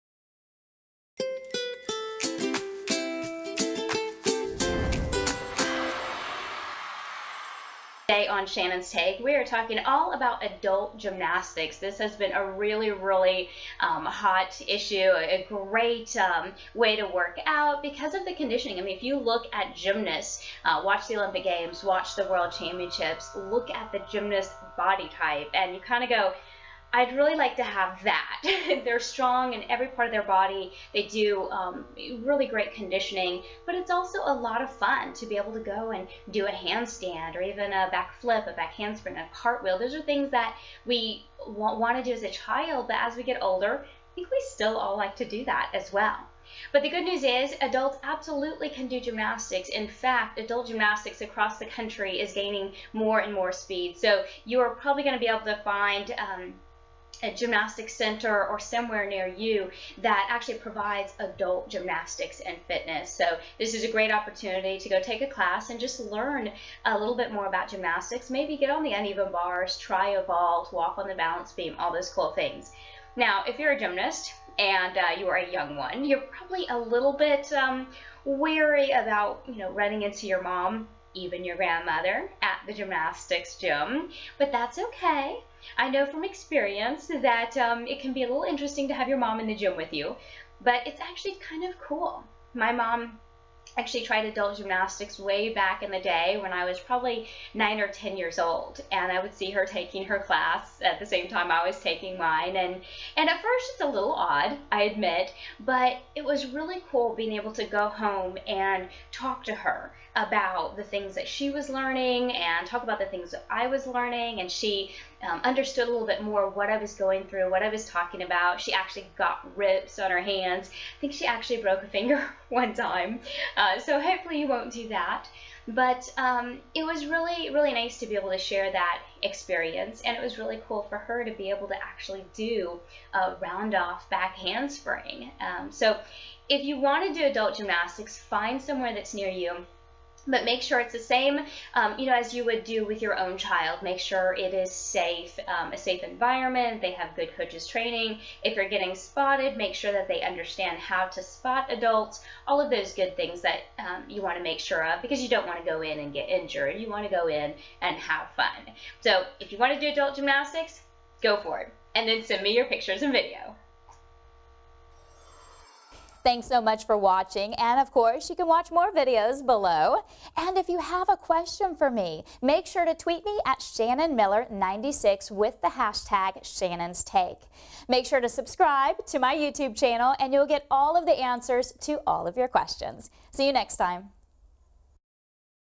ShannonsTake: Can Adults Do Gymnastics?
Shannon Miller answers your questions! This week's question concerns whether or not adults should give gymnastics a shot. You might be surprised to hear Shannon's answer!

Do you have a question for Shannon? Use the hashtag, ShannonsTake and ask - the country's most decorated gymnast may answer your question next!